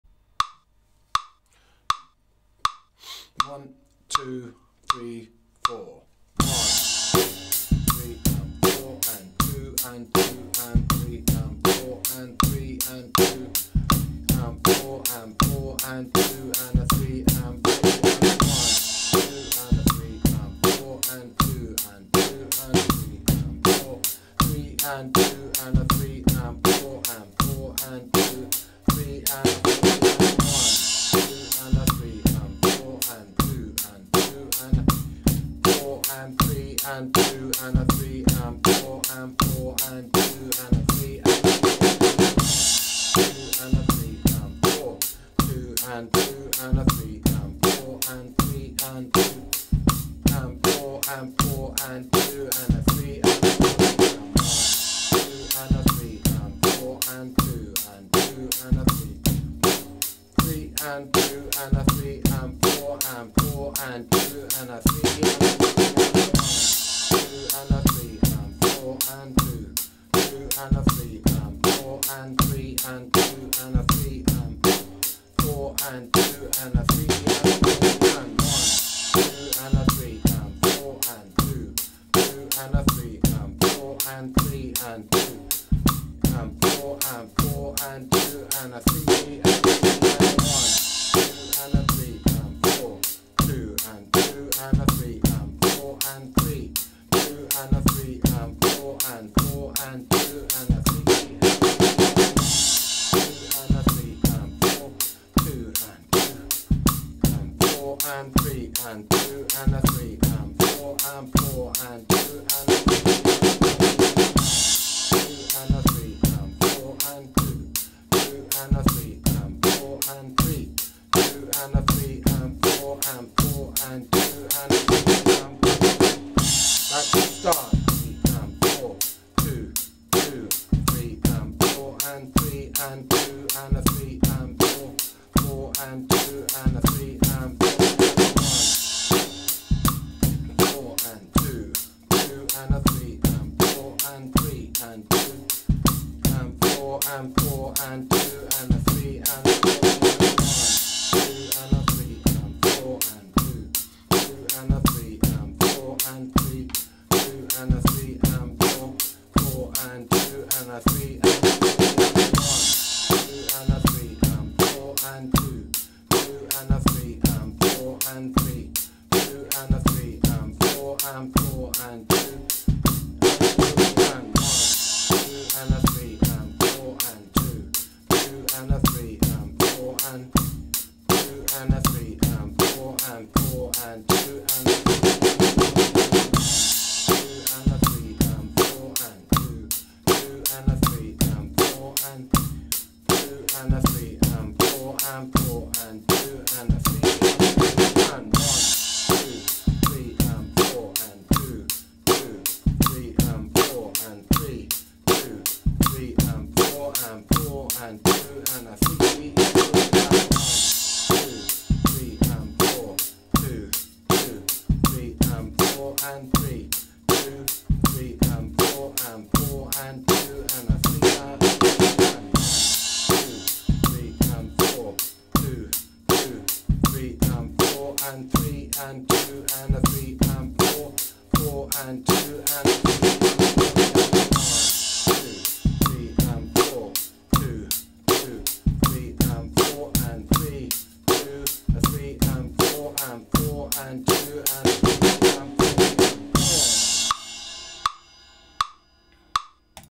Mini Monster Page 58 Fills 1 to 10 In 4 Bar Phrases
Playing through fills 1-10 on P58 of Joel Rothman''s Mini Monster Book of Rock Drumming. Once you can play these in a nice relaxed fashion, try playing them around the kit. Then use these fills when playing along to music.